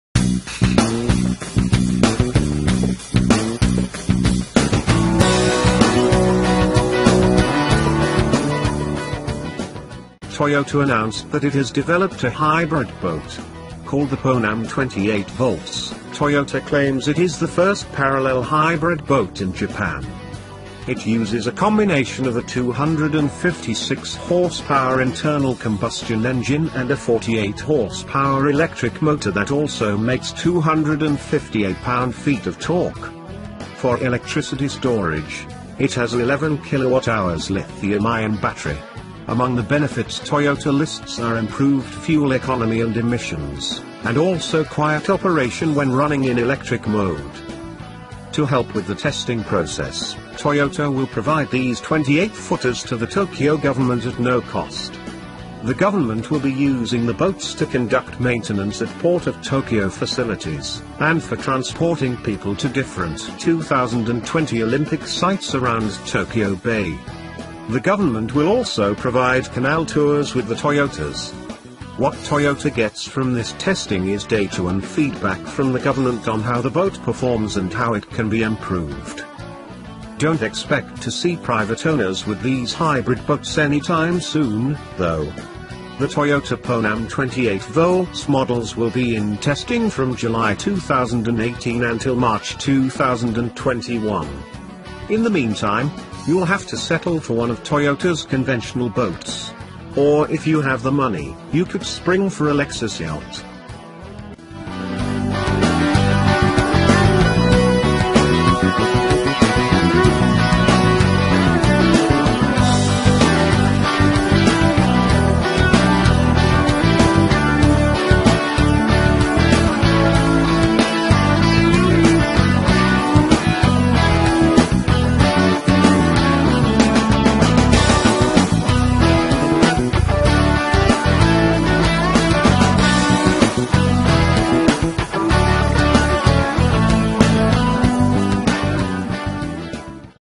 WOW! Toyota's latest hybrid is a boat
Toyota announced that it has developed a hybrid boat. Called the PONAM-28V, Toyota claims it is the first parallel hybrid boat in Japan. It uses a combination of a 256-horsepower internal-combustion engine and a 48-horsepower electric motor that also makes 258 pound-feet of torque. For electricity storage, it has a 11 kWh lithium-ion battery. Among the benefits Toyota lists are improved fuel economy and emissions, and also quiet operation when running in electric mode.